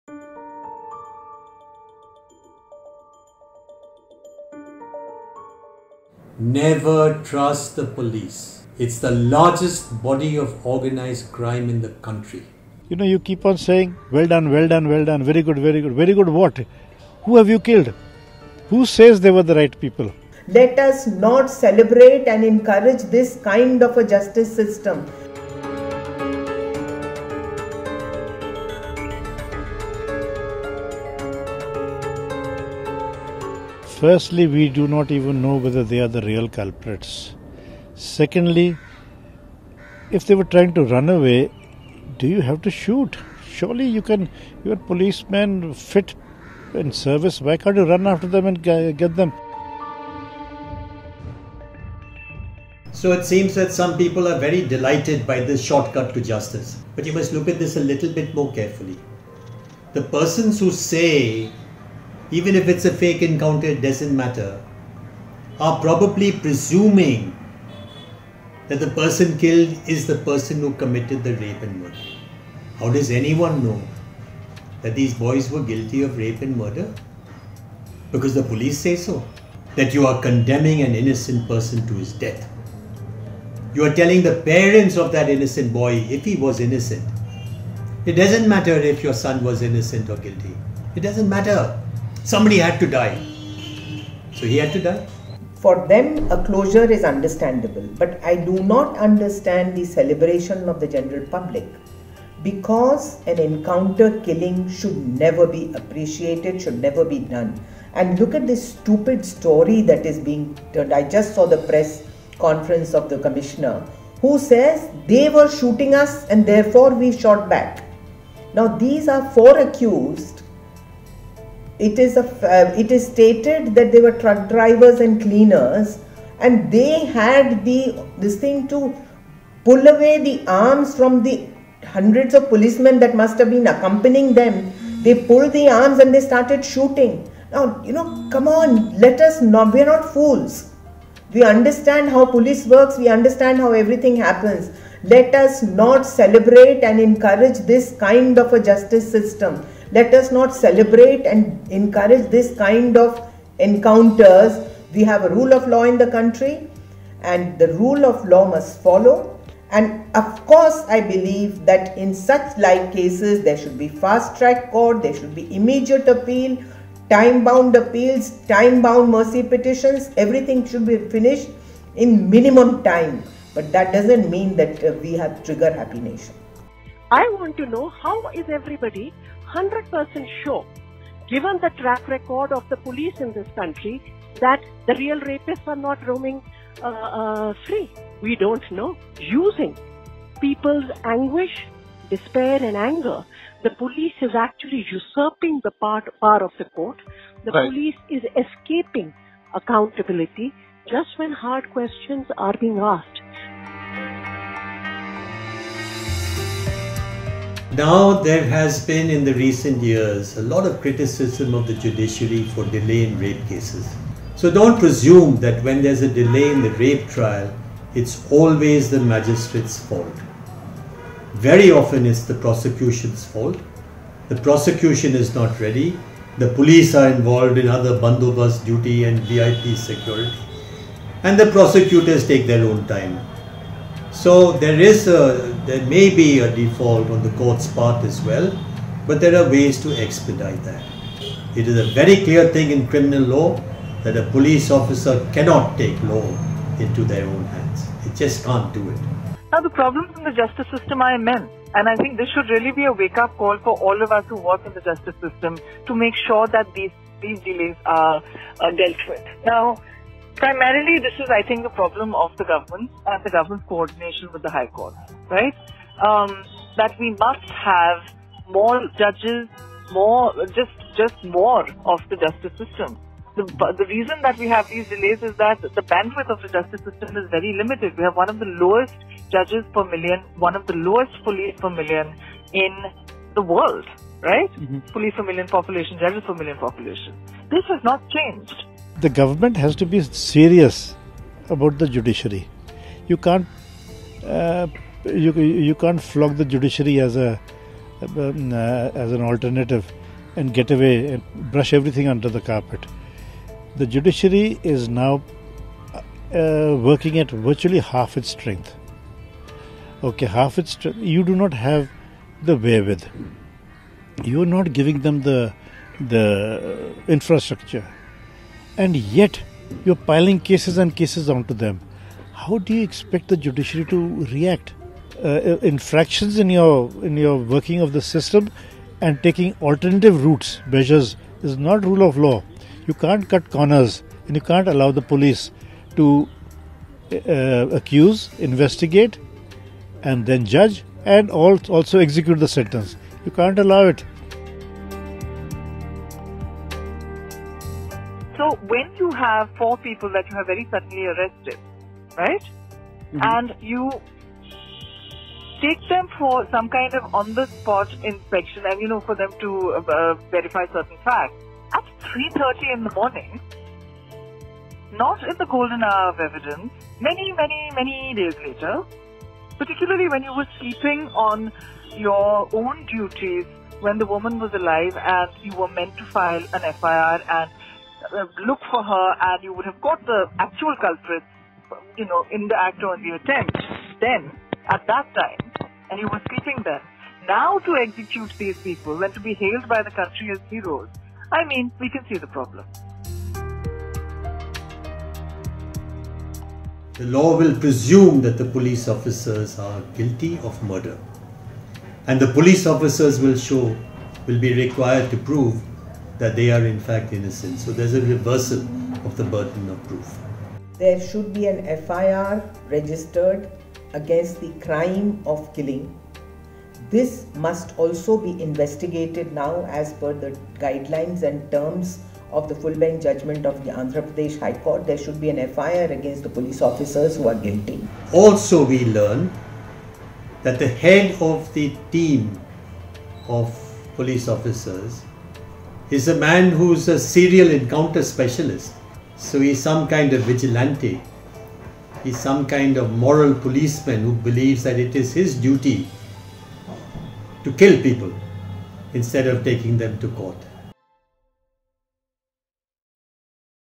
'This Is Not Justice': Senior Lawyers React to Hyderabad Encounter | The Quint
Leading lawyers & activists who have fought numerous cases to uphold women’s rights have slammed Hyderabad encounter and demanded an independent judicial probe into it.